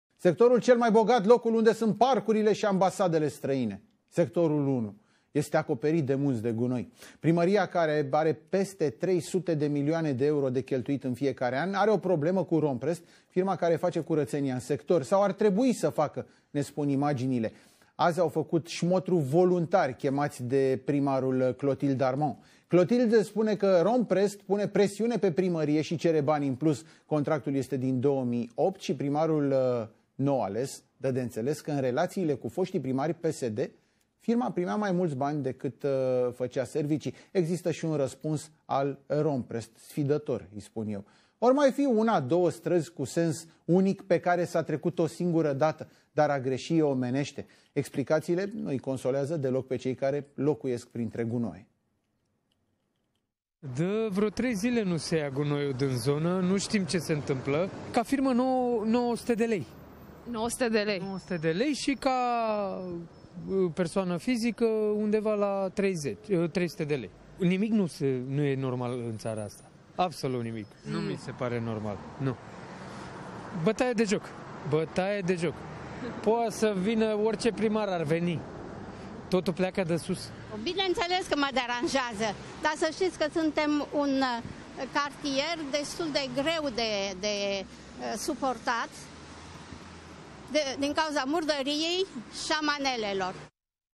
De 3 zile Romprest nu a strâns gunoiul de pe unele străzi din sectorul 1
Ne găsești pe Aleph News !